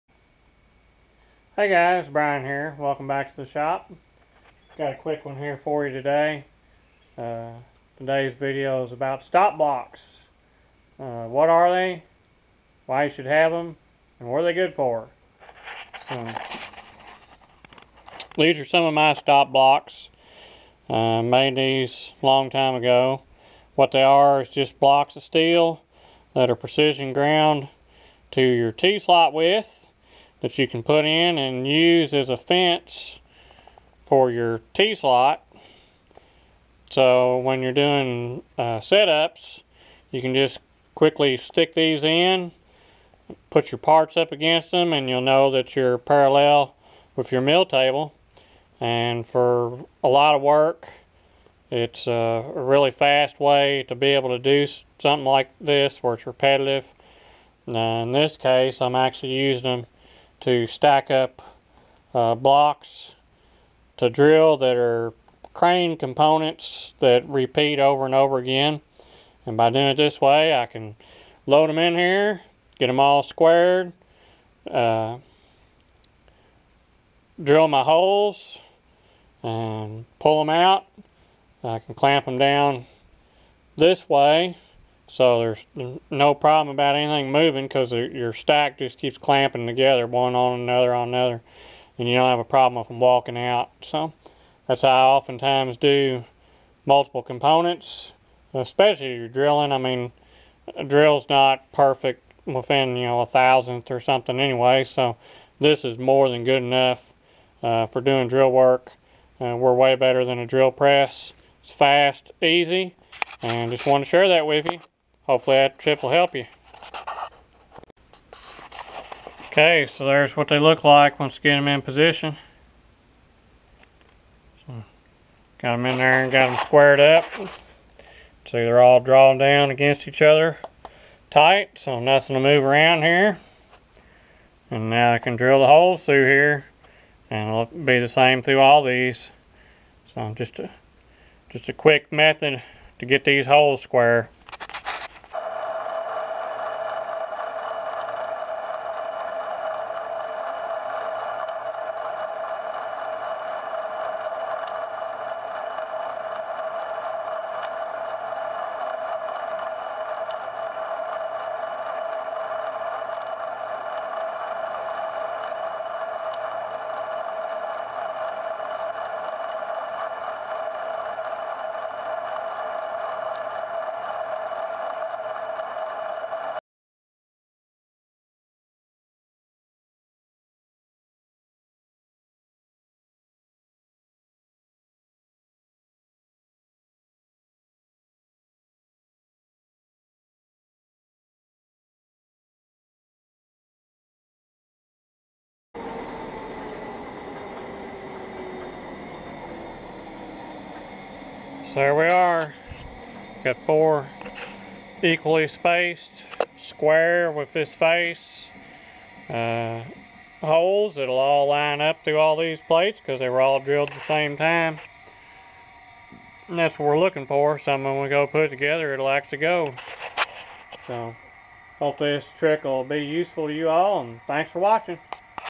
Stop Blocks
Stop Blocks. What are they, why you should have them, and how do you use them. I briefly touched on these in a video before but decided to do one dedicated just to them as using stop blocks is a huge time saver and by providing a rigid fence they give stability to your parts as well so they don't move on you while machining.